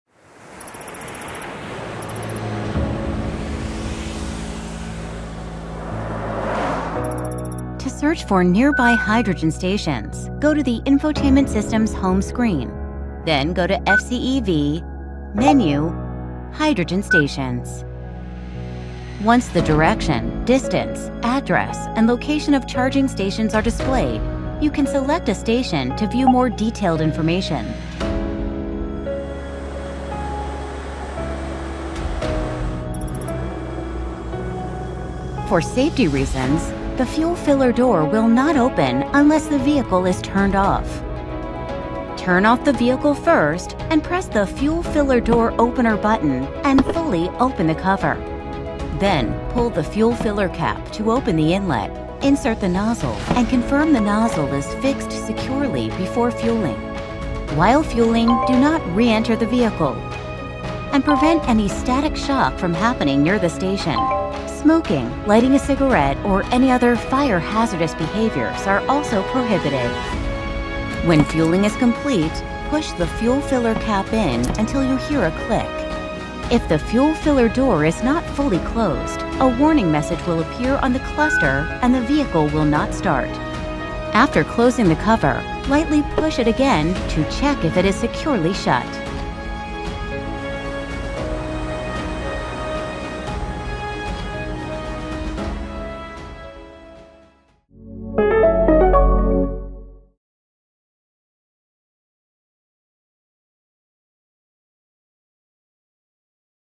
The all-new NEXO | Hydrogen Refueling
The all-new NEXO How-To ― Hydrogen Refueling

Only You Can Make It Happen, The All-New NEXO 🌳
The all-new NEXO, Hyundai’s high-tech hydrogen EV is back after 7 years.
This Walkthrough video will guide you through the new mobility lifestyle you will experience with the new FCEV.

*The specifications shown in the video may differ from the actual sales specifications.
*Computer graphics were used in this video to help understanding. Some figures and shapes may differ from the actual ones.